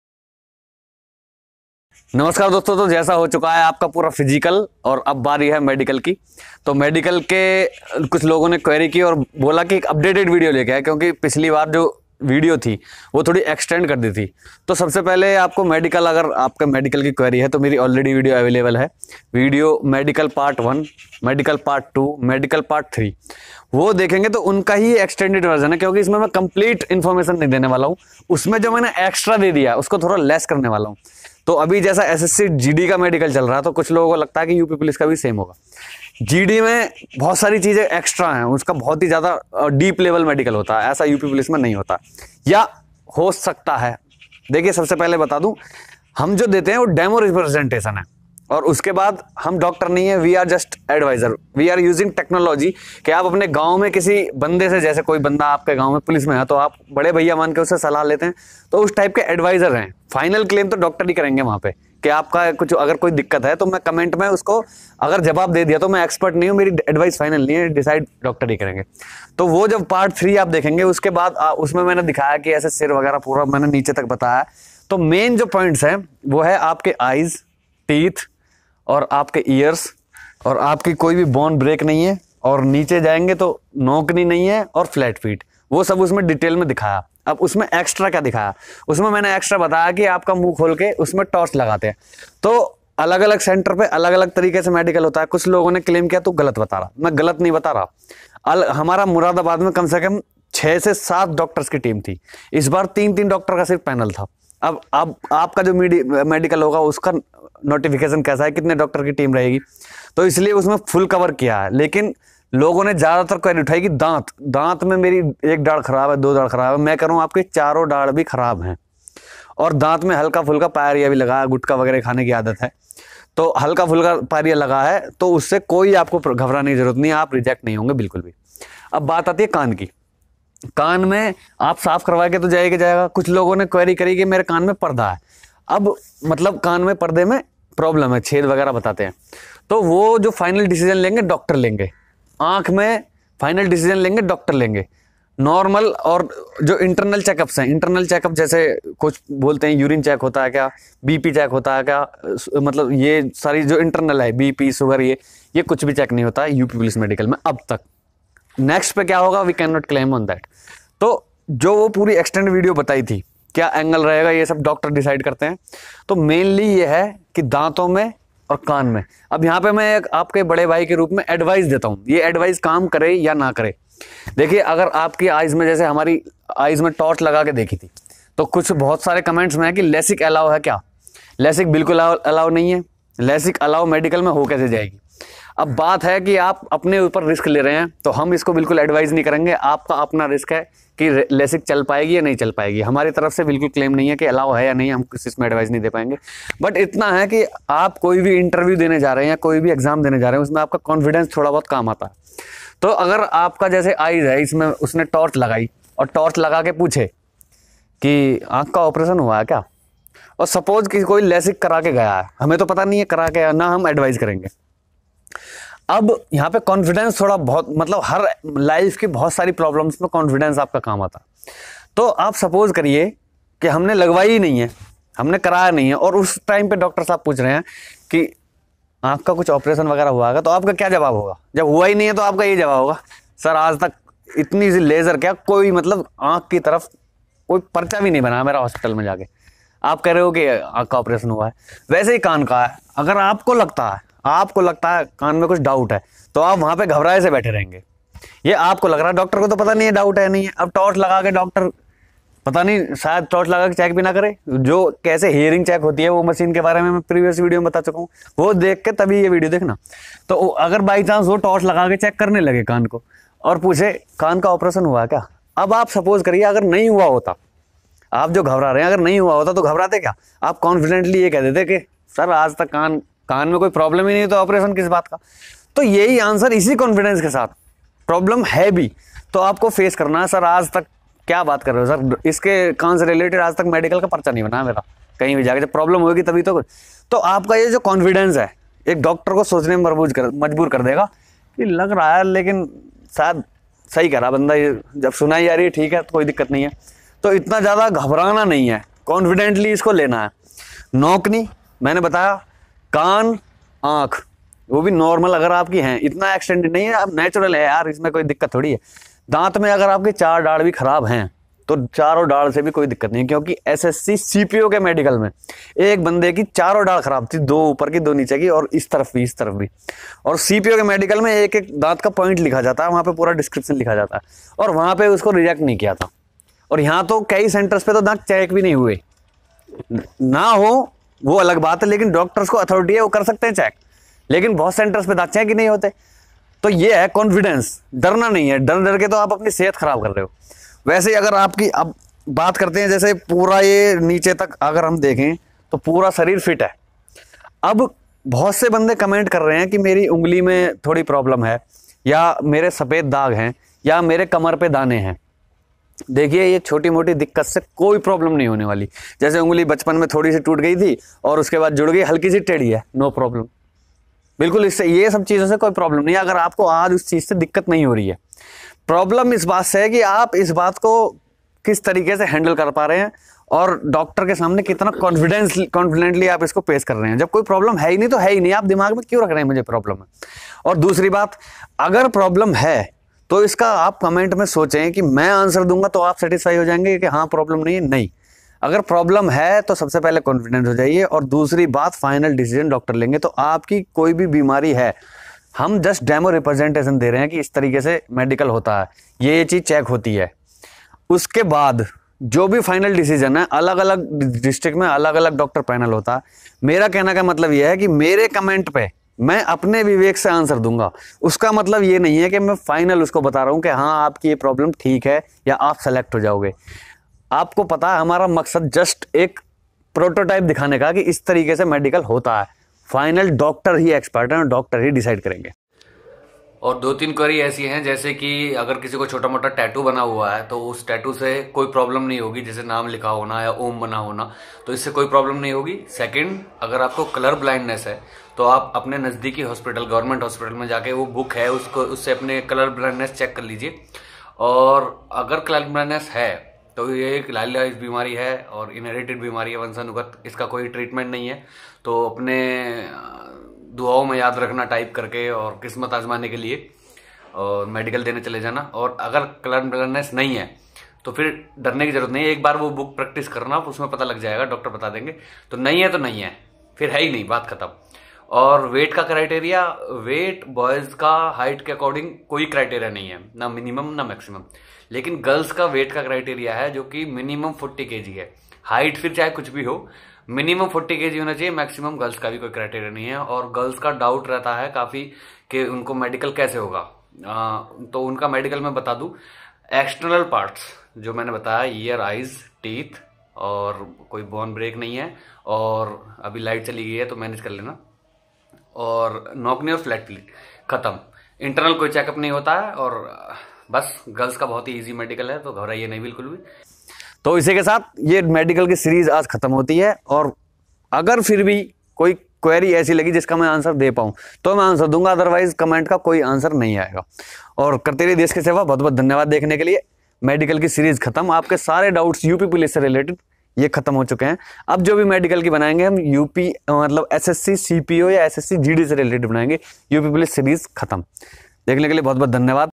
UP police medical test me kya hota h | kaise hota h
UP police constable medical test
UP police medical

This video is a demo representation of UP police constable medical test.
I tried to apply my knowledge and experience of up police constable medical and collect opinion of some new recruits.

This is not an UP govt. authorized channel.
So take is such as an advisory visual.

Thanks and all the best.